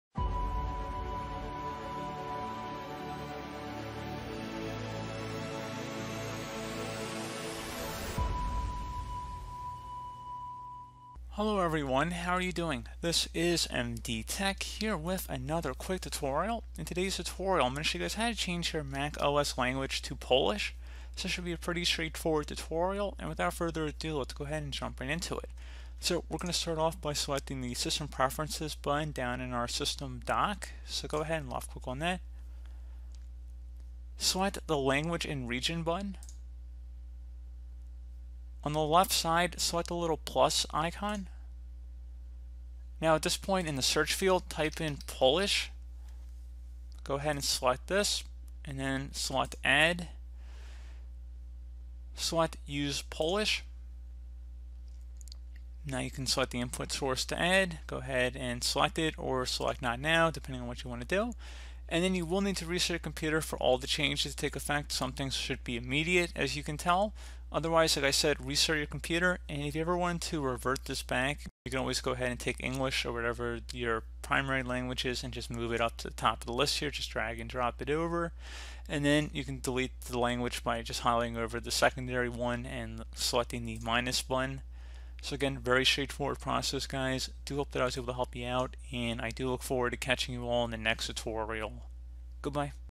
How To Change macOS Language To Polish [Tutorial]
Although your Mac is set to display the language of the country or region in which it was purchased, you can choose a different language to use. For example, if you bought your Mac in the United States but you work primarily in Poland, you can set your Mac to use Polish.

You can also choose different languages for individual apps. For example, if your system language is set to Simplified Chinese, but you prefer to use a certain app in English, you can do so.

This tutorial will apply for MacBook Pro, MacBook Air, MacBook Mini's running the macOS operating system.

Devices covered in this tutorial: macOS, macOS Big Sur, MacBook Pro, MacBook Air, MacBook Mini